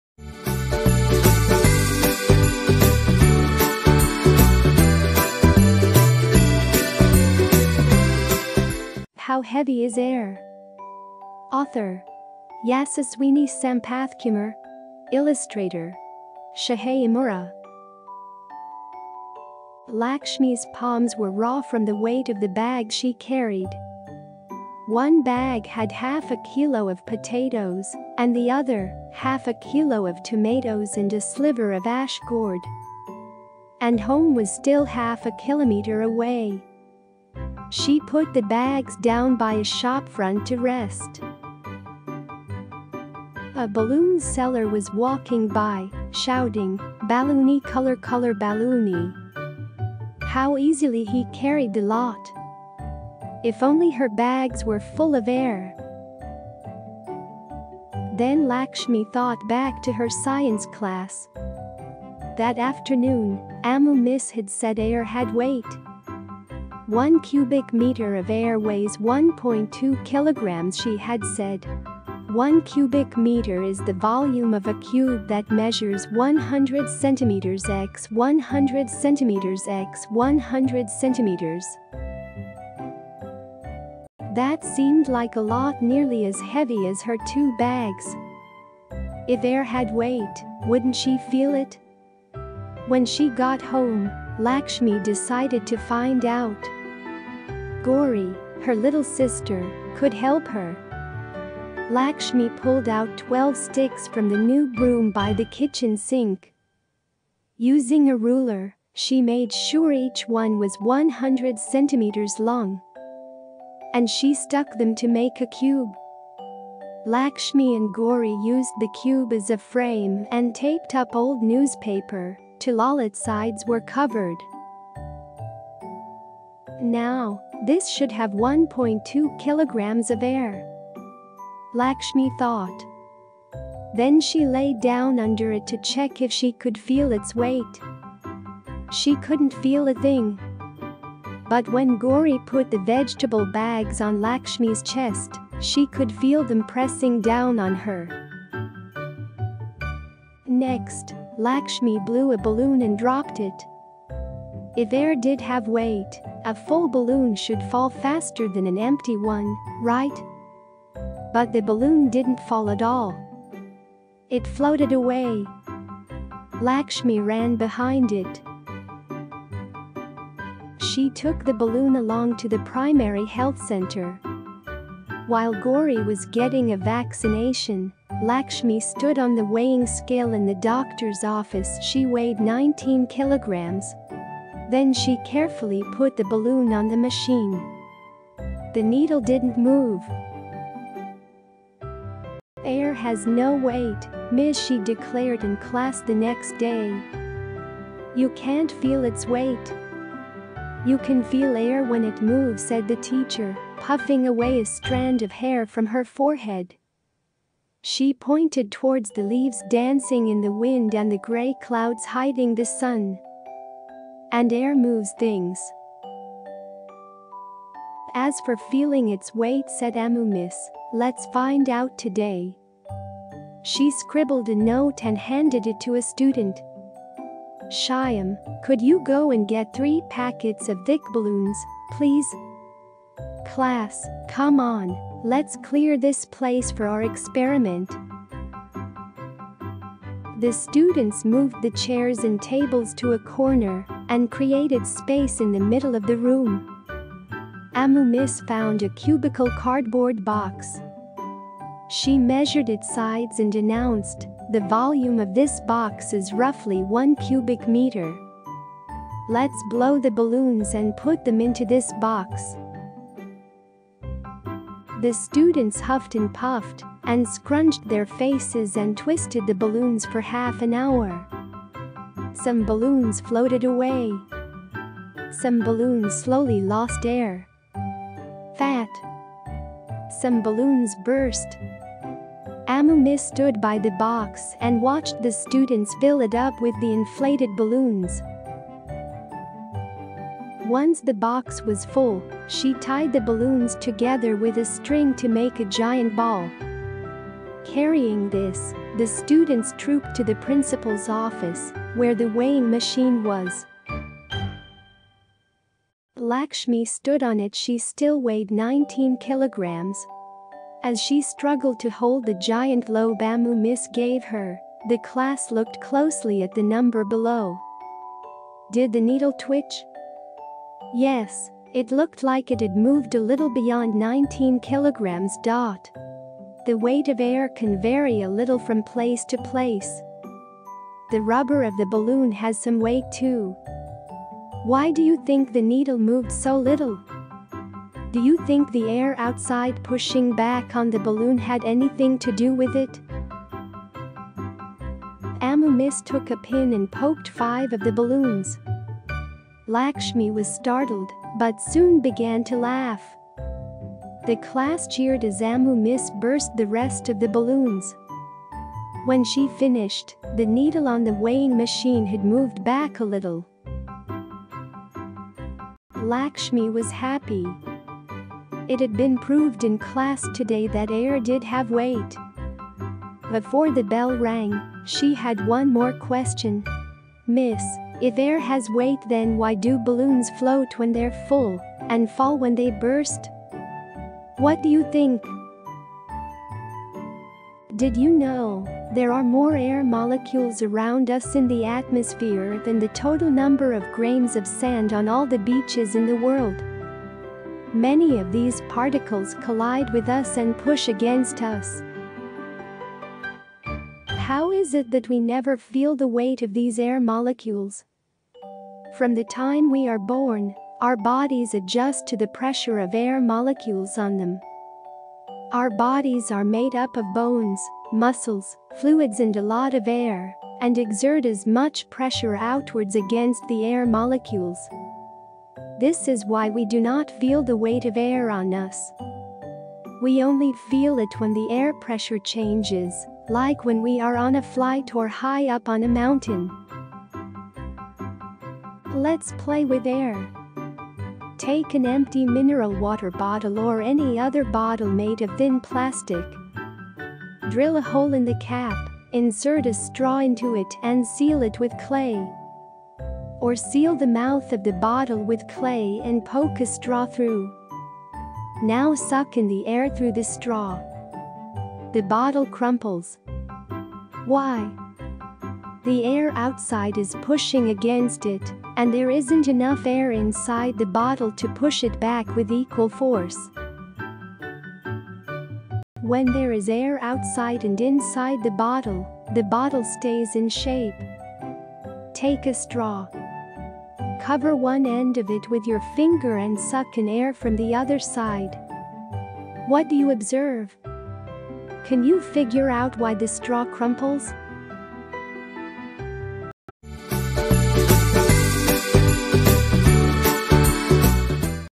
[Vibo4Kids]How Heavy is Air?
How Heavy is Air?
Author: Yasaswini Sampathkumar
Illustrator: Shohei Emura
Lakshmi’s palms were raw from the weight of the bags she carried.
One bag had half a kilo of potatoes, and the other, half a kilo of tomatoes and a sliver of ash gourd.
And home was still half a kilometre away.
She put the bags down by a shopfront to rest.
A balloon seller was walking by, shouting, “Balooooneee – colour-colour baloooonee.”
How easily he carried the lot.
If only her bags were full of air!
Then Lakshmi thought back to her science class.
That afternoon, Ammu Miss had said air had weight.
“One cubic metre* of air weighs 1.2 kg,” she had said. *1 cubic metre is the volume of a cube that measures 100 cm x 100 cm x 100 cm.
That seemed like a lot – nearly as heavy as her two bags.
If air had weight, wouldn’t she feel it?
When she got home, Lakshmi decided to find out.
Gowri, her little sister, could help her.
Lakshmi pulled out twelve sticks from the new broom by the kitchen sink.
Using a ruler, she made sure each one was 100 cm long.
And she stuck them to make a cube.
Lakshmi and Gowri used the cube as a frame and taped up old newspaper till all its sides were covered.
‘Now, this should have 1.2 kilograms of air!’
Lakshmi thought.
Then she lay down under it to check if she could feel its weight.
She couldn’t feel a thing!
But when Gowri put the vegetable bags on Lakshmi’s chest, she could feel them pressing down on her.
Next, Lakshmi blew a balloon and dropped it.
If air did have weight, a full balloon should fall faster than an empty one, right?
But the balloon didn’t fall at all.
It floated away.
Lakshmi ran behind it.
She took the balloon along to the primary health centre.
While Gowri was getting a vaccination, Lakshmi stood on the weighing scale in the doctor’s office – she weighed 19 kg.
Then she carefully put the balloon on the machine.
The needle didn’t move.
“Air has no weight, Miss,” she declared in class the next day.
“You can’t feel its weight.”
“You can feel air when it moves,” said the teacher, puffing away a strand of hair from her forehead.
She pointed towards the leaves dancing in the wind and the grey clouds hiding the sun.
“And air moves things.”
“As for feeling its weight,” said Ammu Miss, “let’s find out today.
She scribbled a note and handed it to a student.
“Shyam, could you go and get three packets of thick balloons, please?
Class, come on, let’s clear this place for our experiment.”
The students moved the chairs and tables to a corner and created space in the middle of the room.
Ammu Miss found a cubical cardboard box.
She measured its sides and announced, “The volume of this box is roughly one cubic metre.
Let’s blow the balloons and put them into this box.”
The students huffed and puffed, and scrunched their faces and twisted the balloons for half an hour.
Some balloons floated away.
Some balloons slowly lost air.
PHAT!
Some balloons burst.
Ammu Miss stood by the box and watched the students fill it up with the inflated balloons.
Once the box was full, she tied the balloons together with a string to make a giant ball.
Carrying this, the students trooped to the Principal’s office, where the weighing machine was.
Lakshmi stood on it – she still weighed 19 kg.
As she struggled to hold the giant globe Ammu Miss gave her, the class looked closely at the number below.
Did the needle twitch?
Yes, it looked like it had moved a little beyond 19 kg*.
*The weight of air can vary a little from place to place.
**The rubber of the balloon has some weight too.
***Why do you think the needle moved so little?
Do you think the air outside pushing back on the balloon had anything to do with it?
Ammu Miss took a pin and poked five of the balloons.
Lakshmi was startled, but soon began to laugh.
The class cheered as Ammu Miss burst the rest of the balloons.
When she finished, the needle on the weighing machine had moved back a little.
Lakshmi was happy.
It had been proved in class today that air did have weight.
Before the bell rang, she had one more question: “Miss, if air has weight then why do balloons float when they’re full, and fall when they burst?”
Did you know, there are more air molecules around us in the atmosphere than the total number of grains of sand on all the beaches in the world!
Many of these particles collide with us and push against us.
How is it that we never feel the weight of these air molecules?
From the time we are born, our bodies adjust to the pressure of air molecules on them.
Our bodies are made up of bones, muscles, fluids and a lot of air and exert as much pressure outwards against the air molecules.
This is why we do not feel the weight of air on us.
We only feel it when the air pressure changes, like when we are on a flight or high up on a mountain.
Let’s play with air!
Take an empty mineral water bottle or any other bottle ...